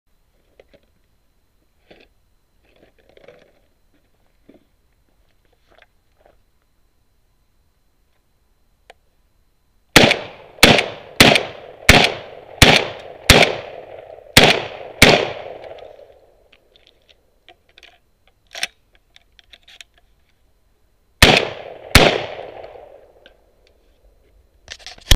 CZ712 Utility 1 1/8 oz Test Extension Tube
Testing the Stoeger M2000 with different shotshell loads before and after installing an extension tube.
This is 1 1/8 oz Remington Sureshot